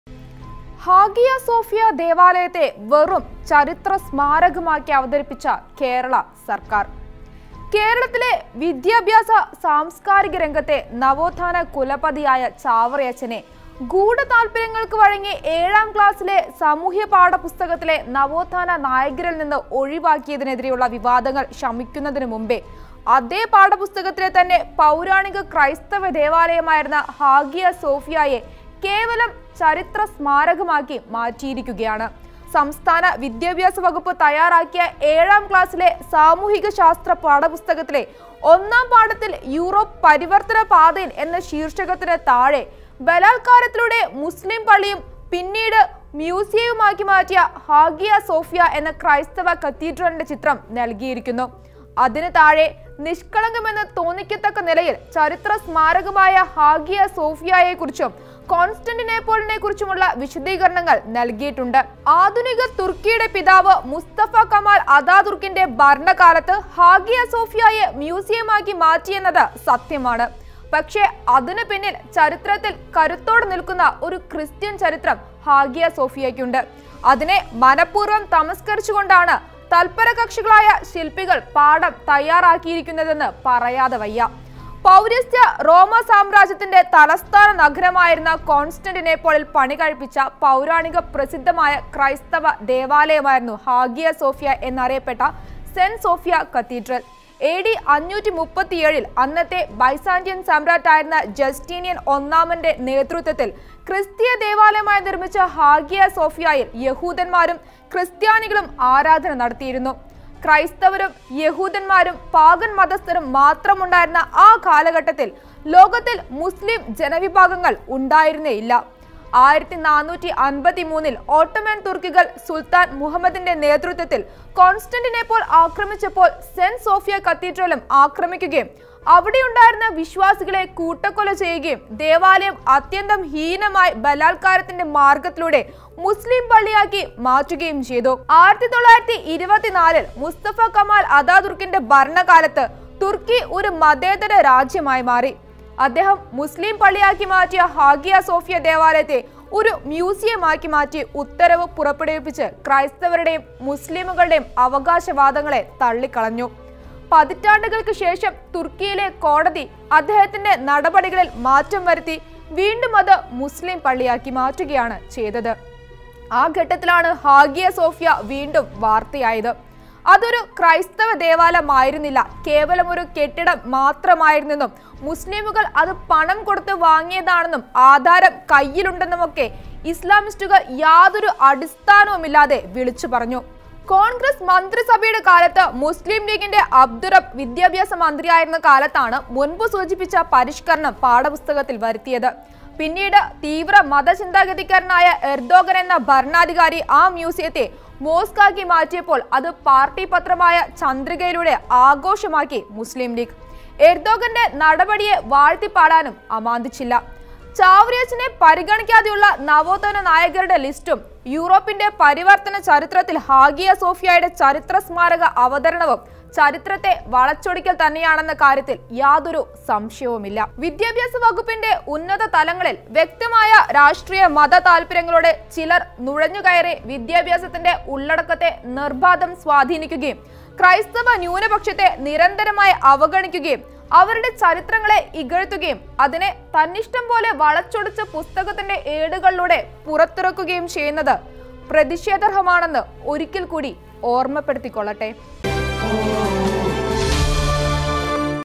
ഹഗിയ സോഫിയാ ദേവാലയത്തെ വെറും ചരിത്രസ്മാരകമാക്കി അവതരിപ്പിച്ച കേരള സർക്കാർ
Idukki Vision  Channel No:51
Bhoomika :52
Malanad Vision  :56